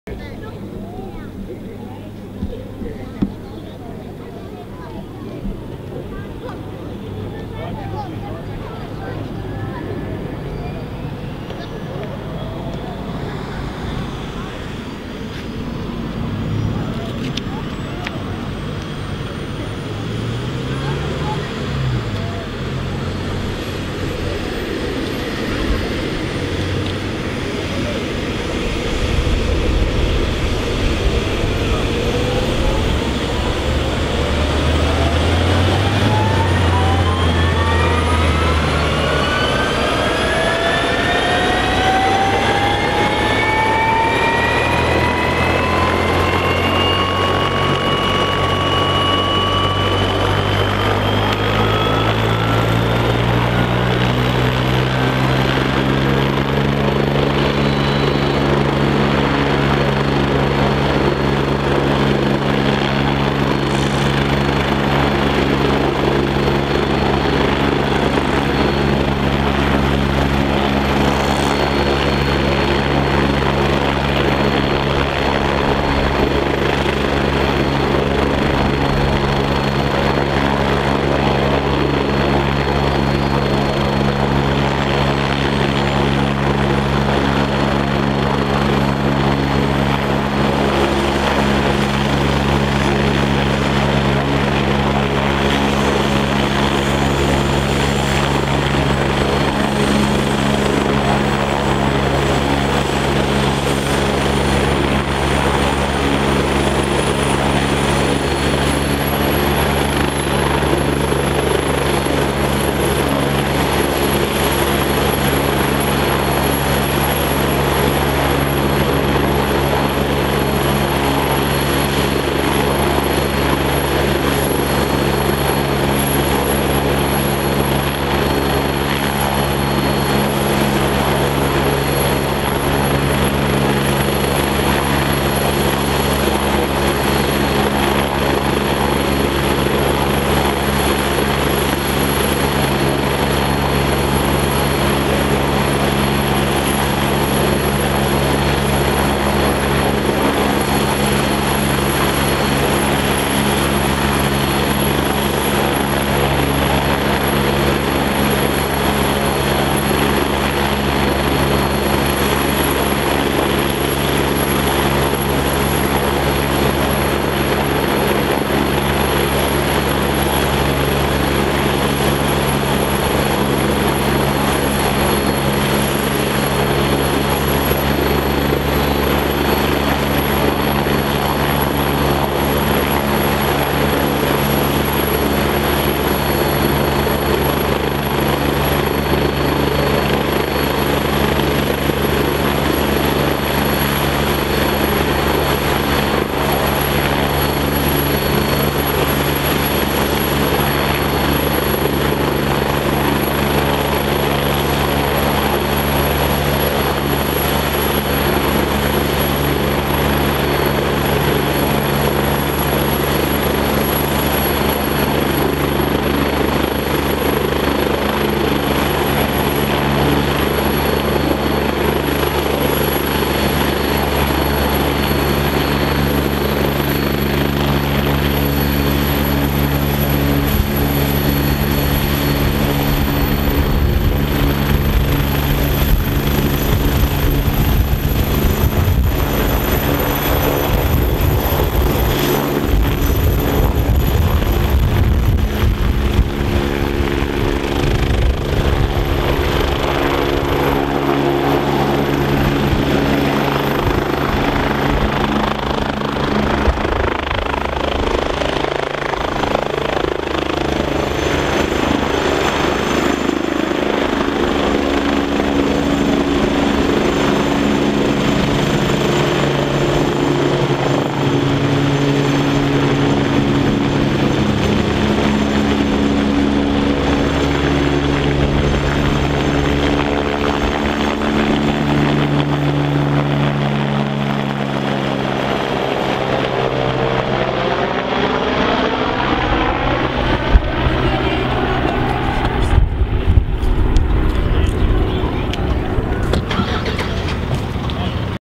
Careflight Helicopter taking off at Springwood Highschool
A young girl was injured, needed to be airlifted to hospital.
3:45 for take off ;)